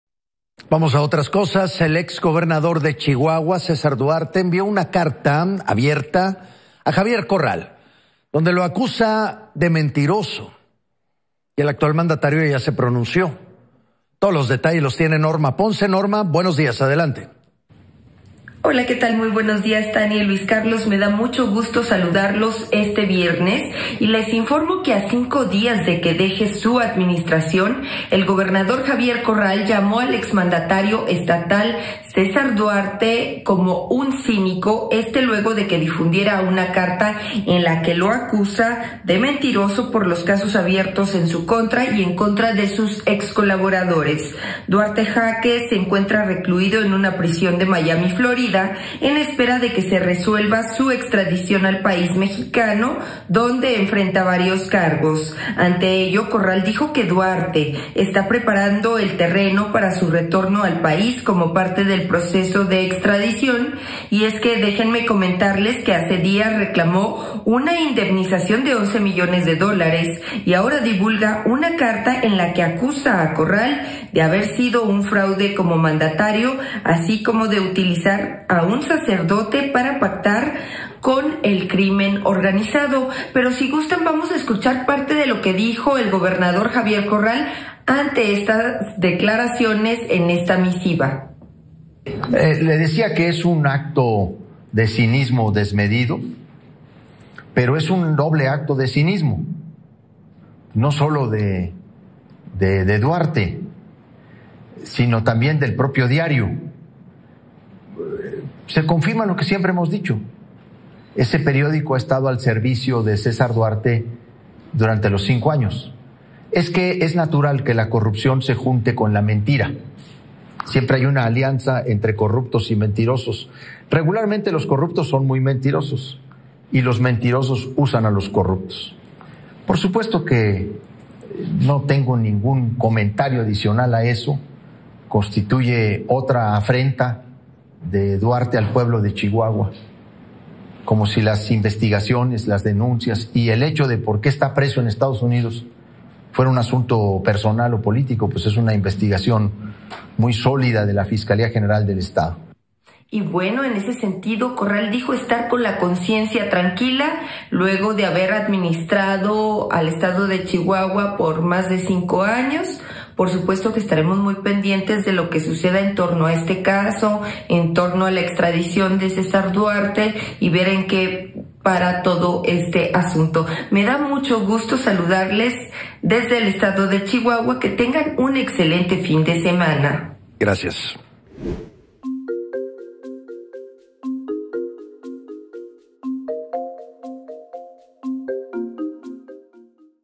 Corral llama "cínico" a Duarte en carta
El ex gobernador de Chihuahua César Duarte envió una carta abierta a Javier Corral donde lo acusa de mentiros, donde el actual mandatario le contesto.